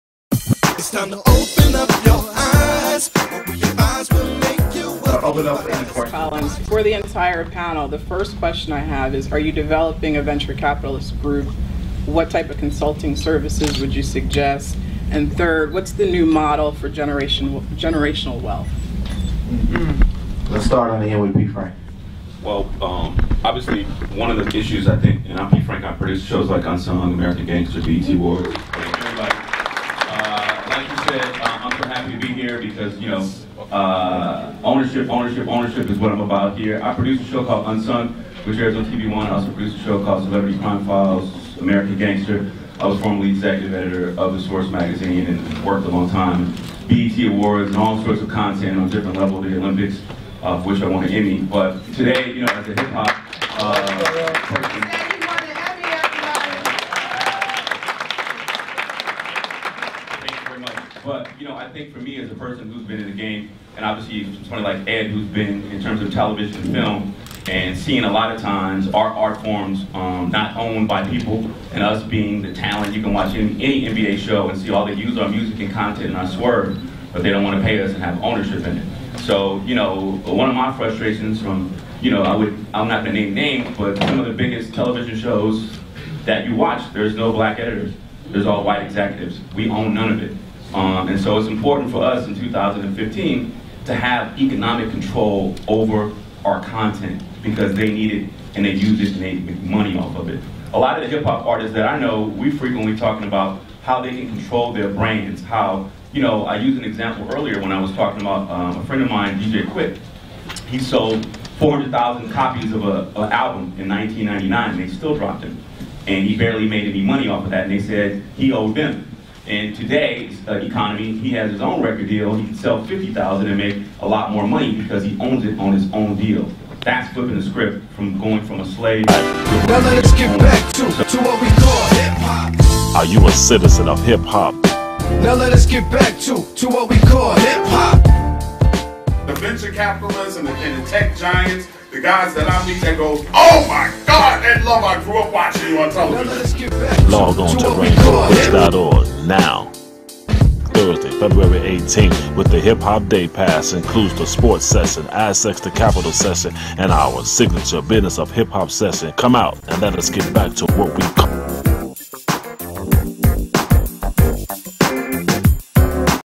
America's Enterprise Hip Hop Questions by Host Erica Collins Answered?
Will the Wall Street Project answer Hip Hop's biggest questions proposed by America's Enterprise Host Erica Collins at the 19th Annual Rainbow Push Economic Summit Join us to find out @EnterpriseECAA